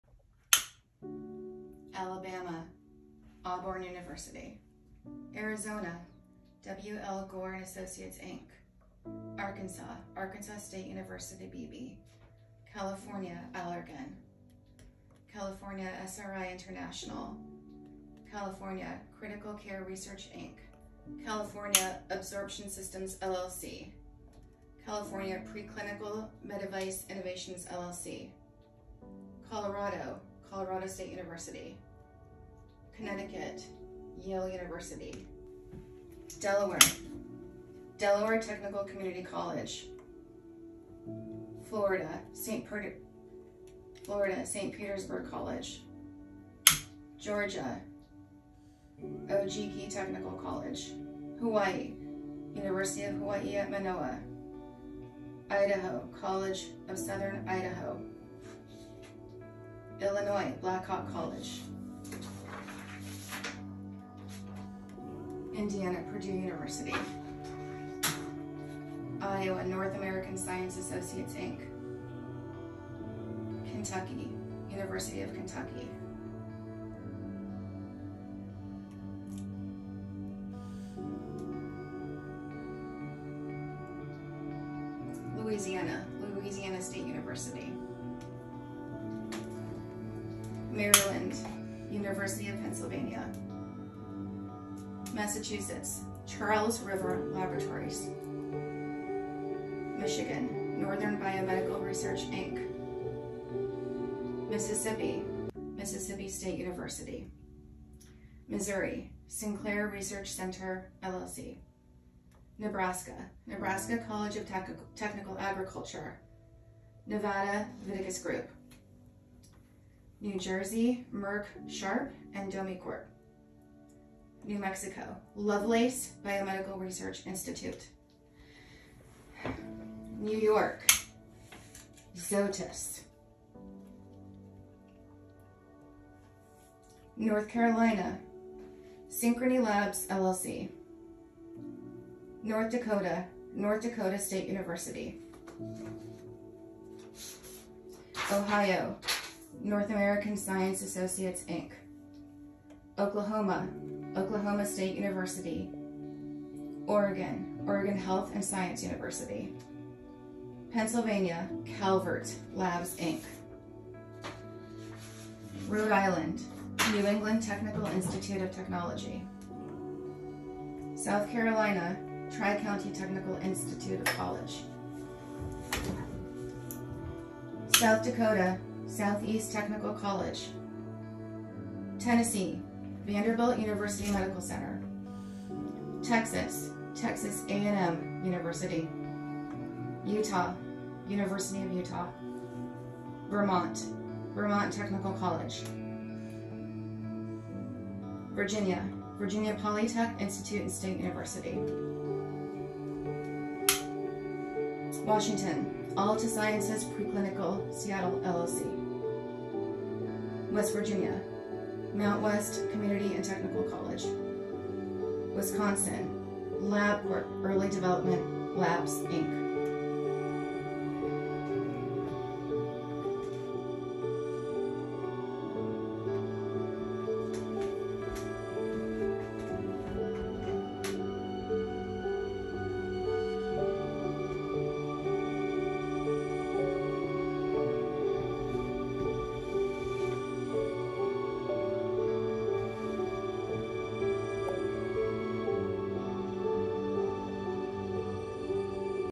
Candlelight vigil for World Week for Animals in Labs
For World Week for Animals in Laboratories we remember and we honor all of the animals who have passed on and those who continue to suffer in laboratories needlessly. This evening at Beagle Freedom Headquarters where we have the honor of liberating so many of these beautiful souls, we are lighting a candle for every state in the U.S. where there are dogs suffering in laboratories right now. We are calling out the name of the specific laboratory in each state as well, and while there are many more labs, we simply cannot name them all. This week is not just a week of remembrance, but a week of calling out the systemic torture of animals and letting these facilities know that we will no longer tolerate this abuse!